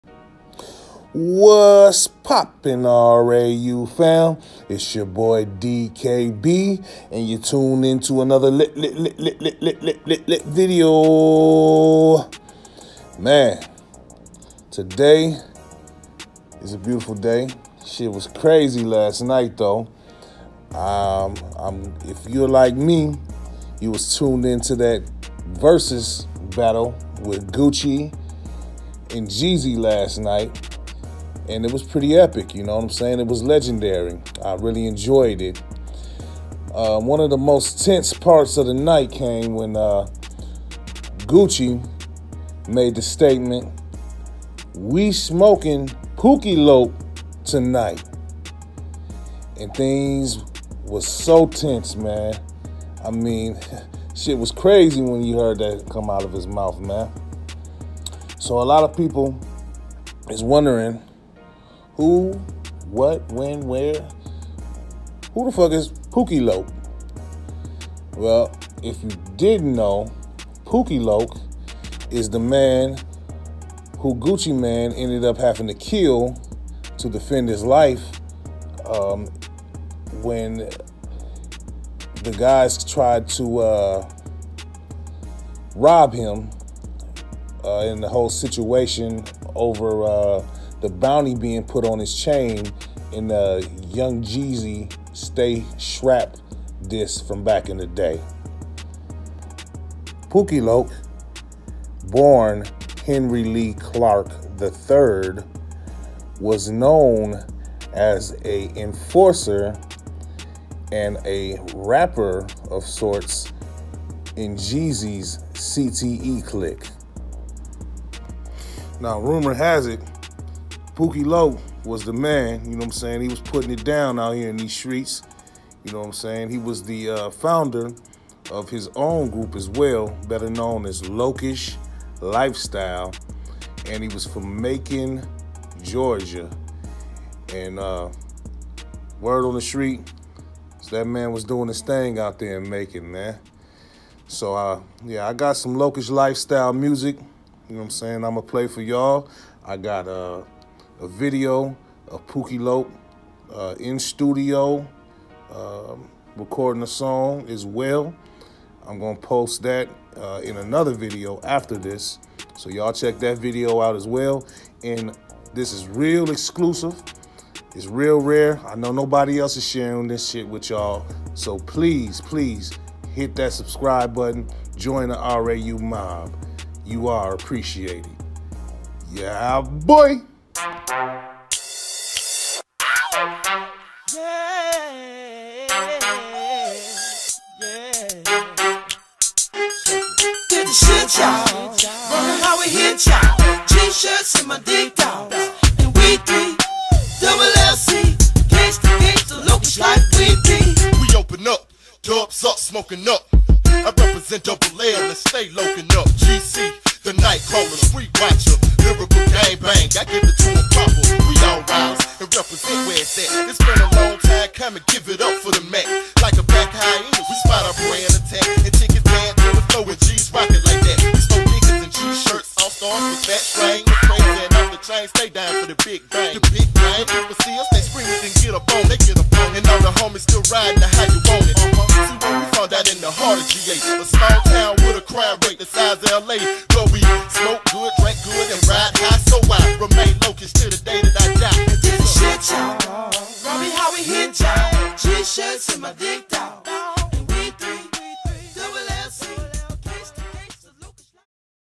Who Is Pookie Loc True Story of The Man Killed in Altercation with Gucci Mane (Exclusive Content)
WHO IS POOKIE LOC?

Pookie Loc – real name Henry Lee Clark III – was a rapper and member of Jeezy’s crew. He was shot dead on May 10th 2005 and Gucci Mane was accused of killing him. Pookie was 27-years-old.
Heavy highlights that Gucci was involved with an altercation with four men inside the home of a friend on this date. One of the men was Pookie and Gucci told investigators that they barged into the apartment and began assaulting people inside.

This led to gunfire and Pookie died, later to be found in a wooded area near Columbia Middle School.

Gucci then turned himself in, admitting to shooting the group of men but adding that his actions were in self-defence.

The charges against Gucci were later dropped by prosecutors in 2006 due to insufficient evidence.
Rip Henry Lee Clark lll